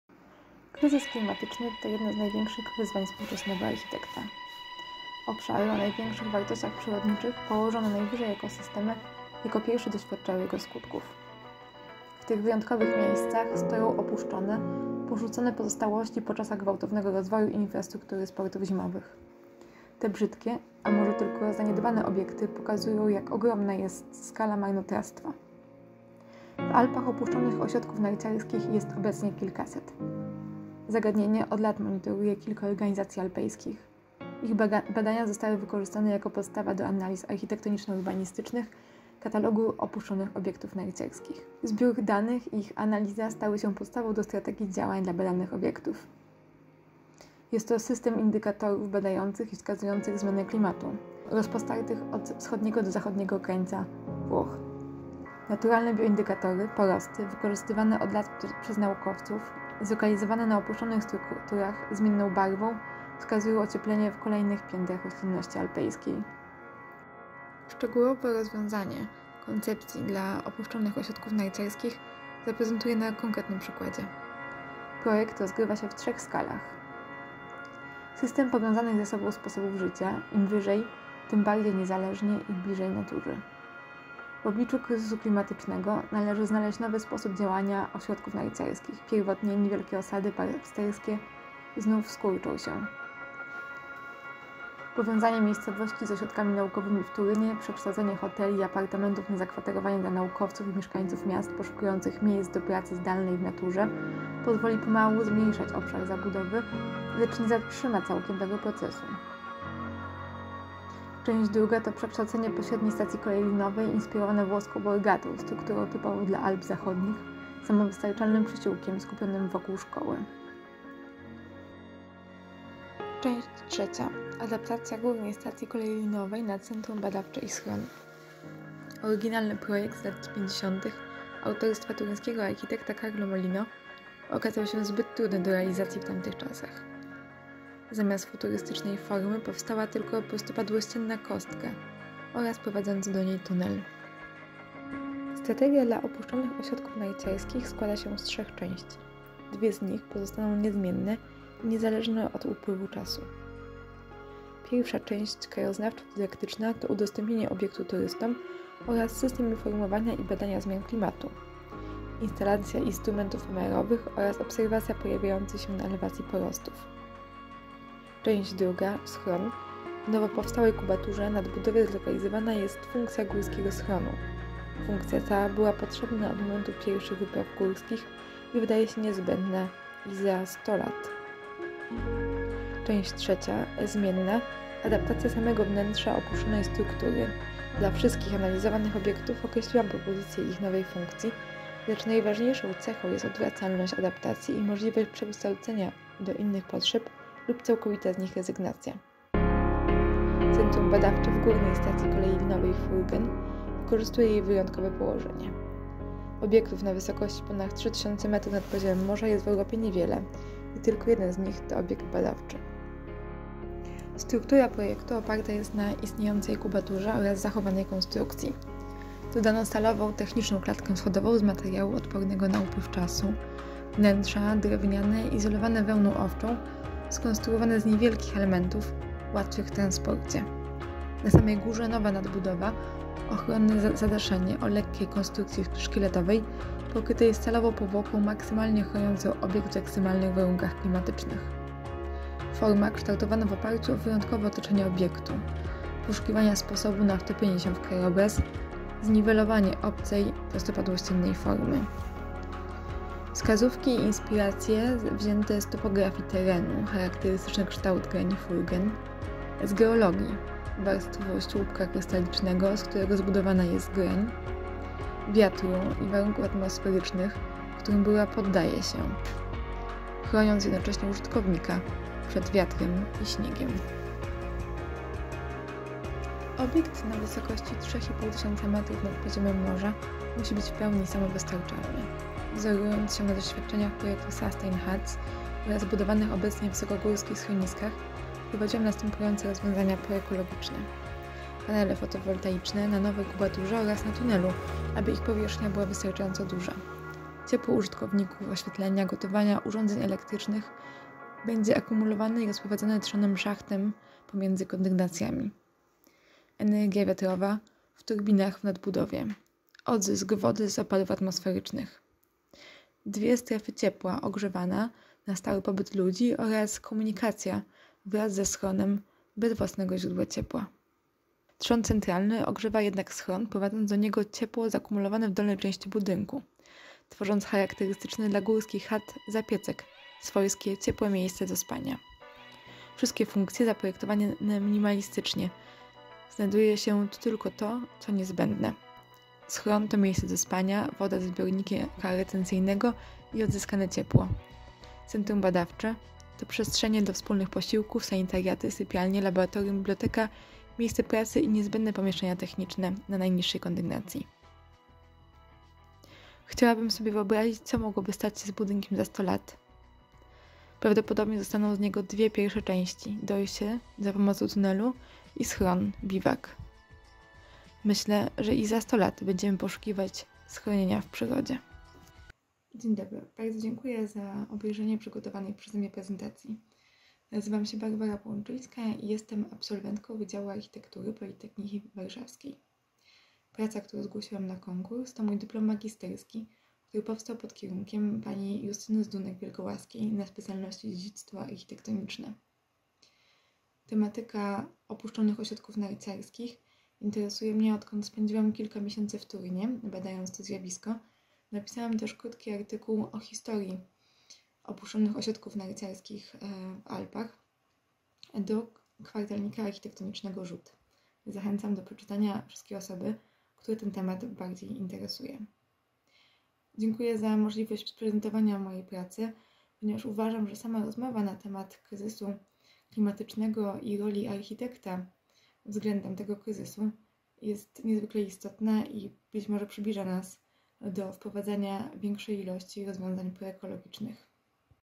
Architektura kryzysu klimatycznego rewitalizacja opuszczonych ośrodków narciarskich w Alpach
ODDAJEMY GŁOS LAUREATOM KONKURSU DLA MŁODYCH ARCHITEKTÓW

Zapraszamy do obejrzenia filmowej prezentacji

Prezentujemy Laureatów Konkursu dla Młodych Architektów w zadaniu Wyzwanie Młodego Architekta

 III NAGRODA

Barbara Płonczyńska
Wydział Architektury Politechniki Warszawskiej
Politechnika Warszawska